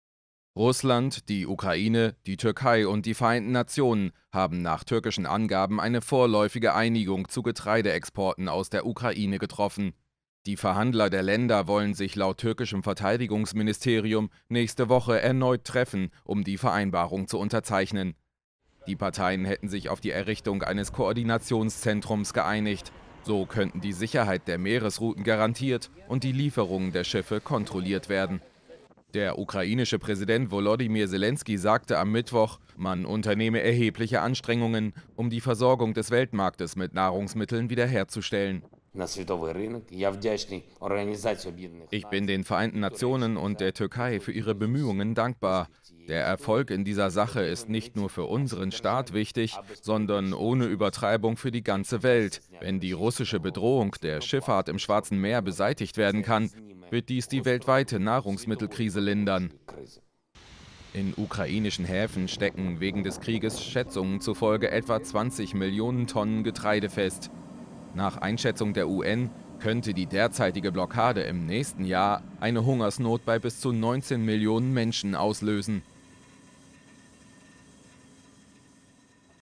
Einigung über Getreideexporte nach Istanbul-Gesprächen in Aussicht
Bei den Gesprächen um die Getreideexporte wurde nach türkischen Angaben eine Einigung erzielt. Man habe sich auf die Errichtung eines Koordinationszentrums geeinigt.

Bildrechte Thumbnail: Miguel Medina/AFP/Getty Images